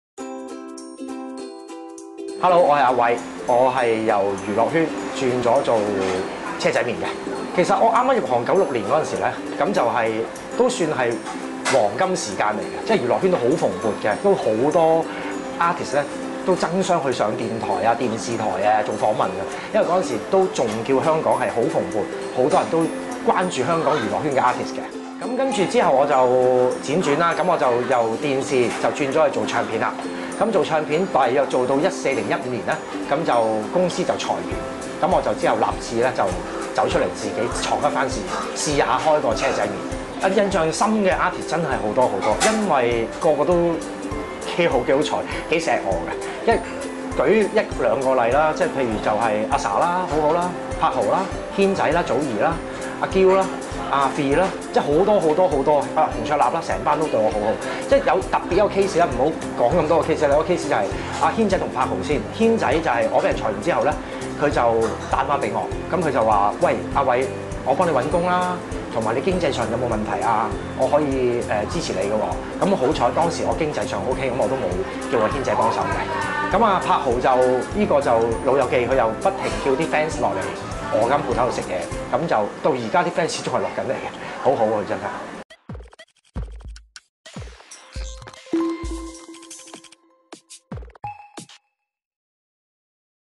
站在星光背後的韋仔 從娛樂圈走到車仔麵
每一個藝人的成功，背後總有很多人默默地努力付出，就像曾經在唱片及電視圈發展的韋仔，多年來一直為旗下藝人落力宣傳，不少藝人都是他的朋友，只可是韋仔最終仍是敵不過唱片業低迷不振，為了自己為了下一代著想，韋仔毅然投入飲食界開車仔麵，不少圈中藝人好友都落力為這位前戰友撐場，平民車仔麵即時「升呢」星級車仔麵……